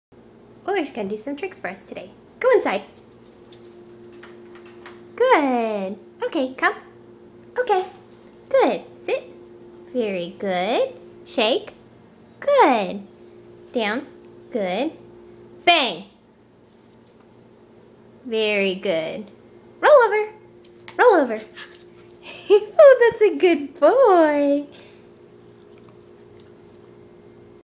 Euler the smart Yorkie!
Euler my 9 month old baby doing tricks!